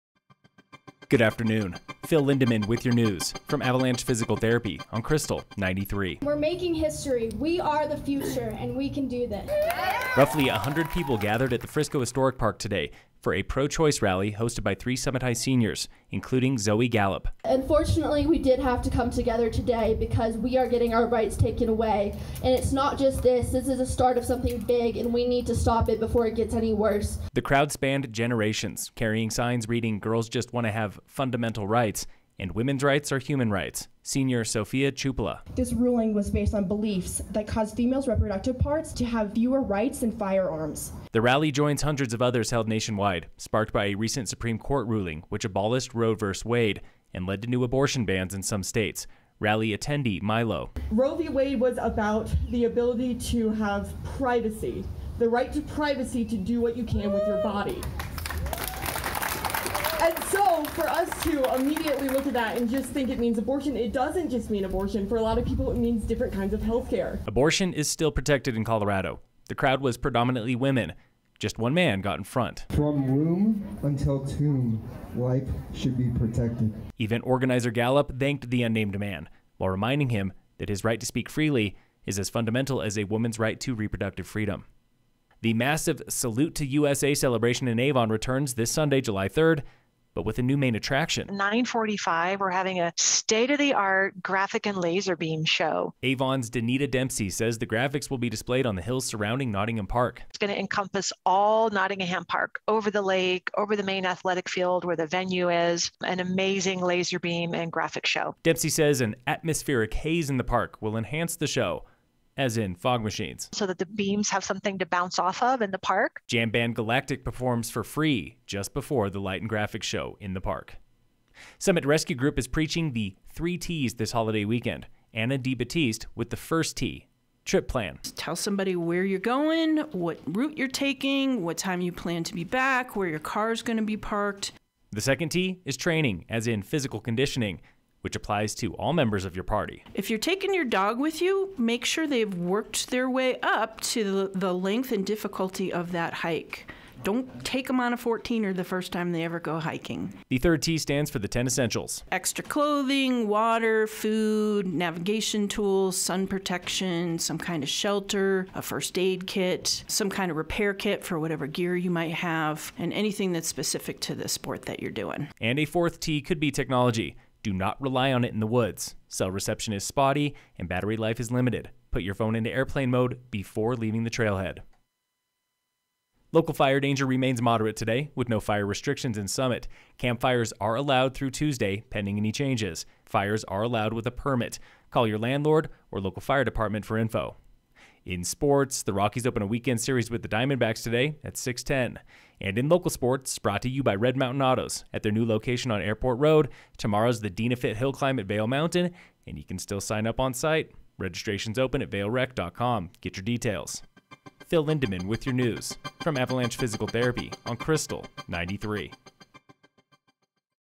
'We're making history': Local teens lead 100 at pro-choice rally in Frisco... on Krystal 93
SEGMENTS
0: -- Highlights from pro-choice rally in Frisco protesting recent Roe v Wade decision
0: -- Avon trades fireworks for laser-light shows this Fourth of July
-- Rescue pros: Practice the Three T's in the wilderness
-- Local fire danger and permits for July 1
-- National sports
-- Local sports

Preview image from pro-choice rally in Frisco, CO on July 1, 2022.